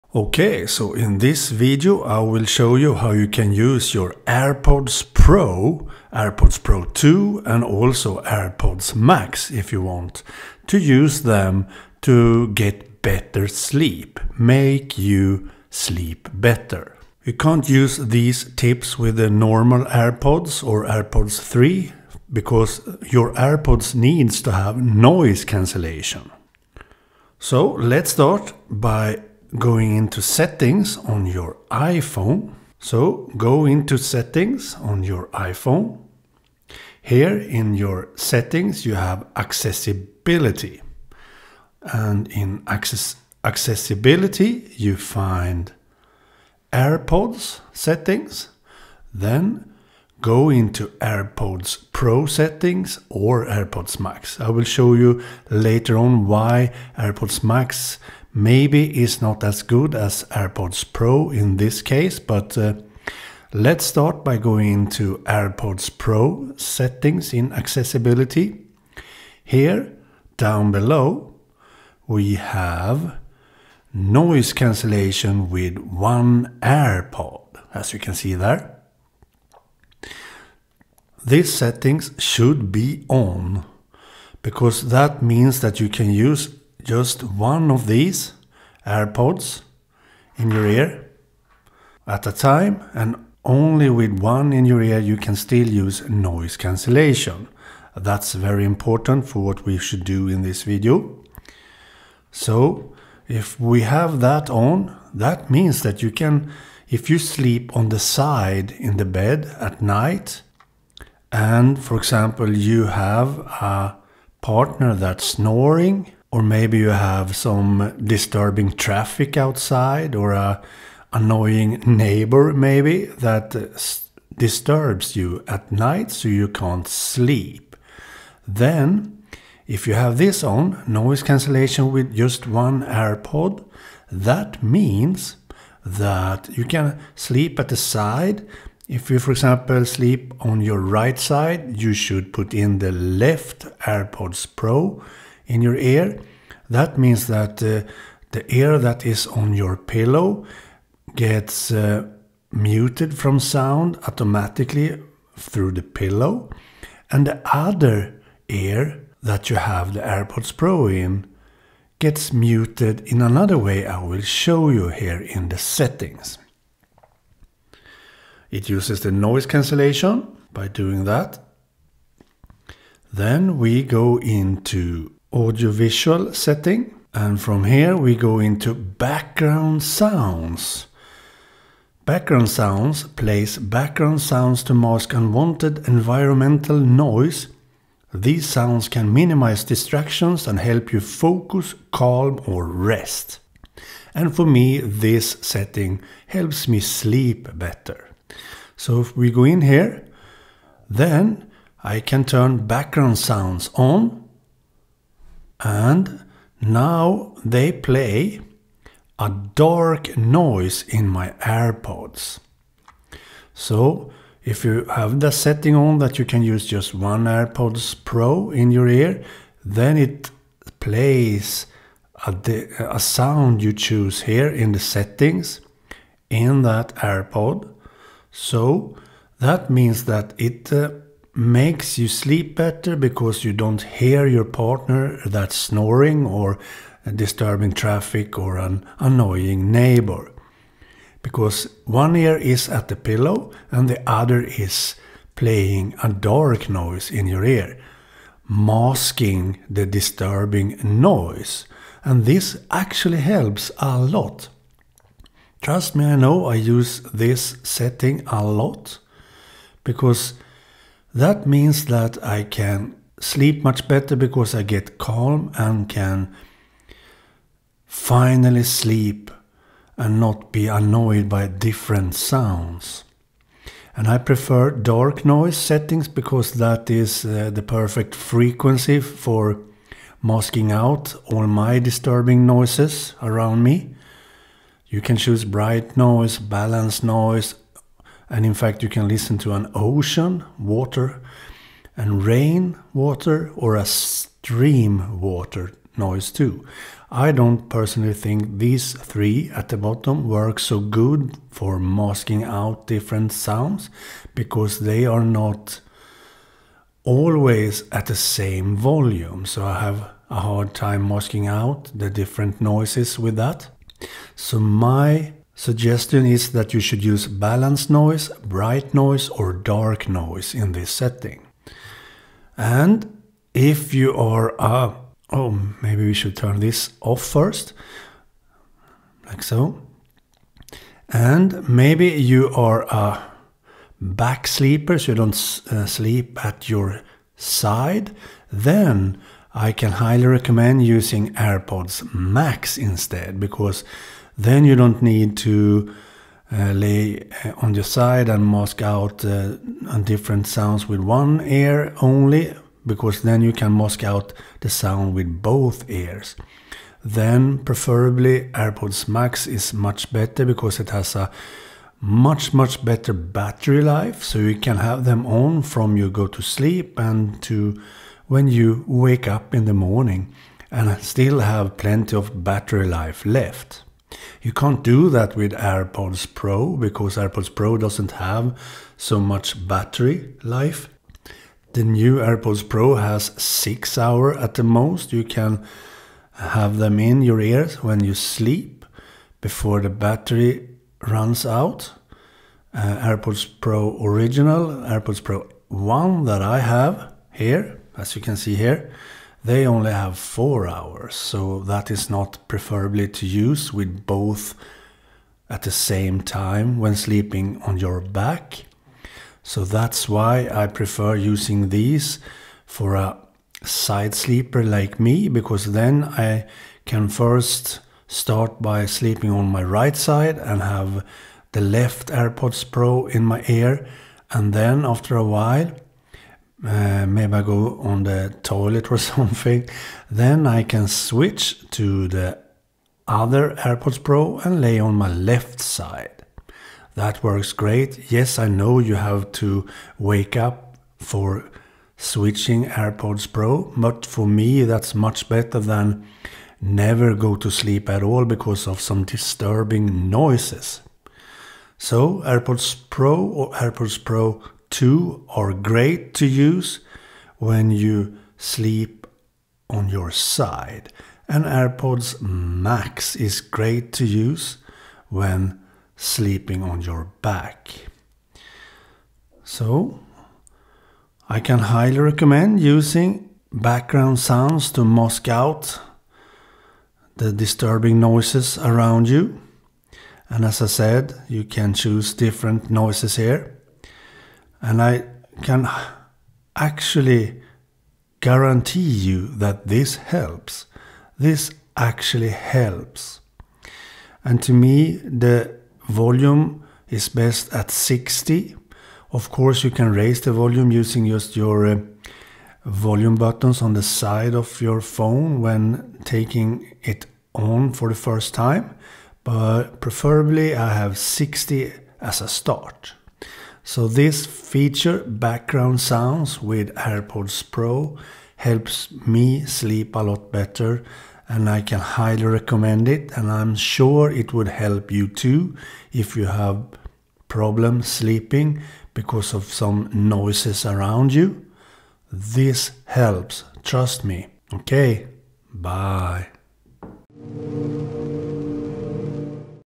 How To Sleep Better Using AirPods Pro as a side-sleeper or AirPods Max if you are a back-sleeper
Here is a tutorial on how to use AirPods Pro or AirPods Max (depending on if you are a side-sleeper or back-sleeper) to help you sleep better. The tutorial uses iOS Accessibility features for using noise cancellation with just on AirPod and Background Noise settings. You can easily also add the background noise feature in the control center menu to easily set it on or off. This actually helps you sleep much better. It really works if you have problems sleeping. It obviously doesn't work with normal AirPods or AirPods 3 because they don't have noise cancellation. I use this feature a lot. Almost every night.